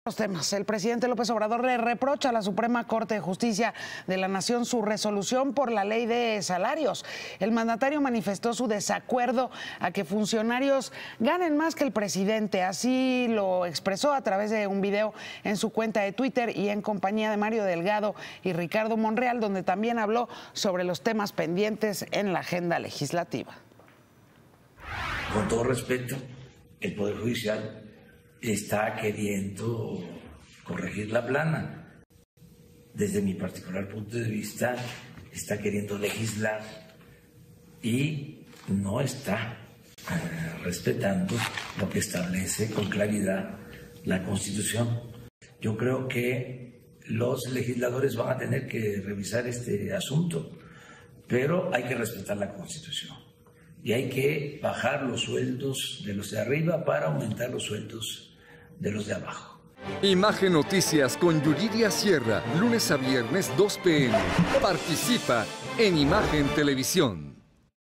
AMLO reprocha a la Suprema Corte su resolución para que funcionarios ganen más que el presidente
A través de un video en su cuenta de Twitter, el presidente Andrés Manuel López Obrador reprochó a la SCJN su resolución para que funcionarios ganen más que el jefe del Poder Ejecutivo.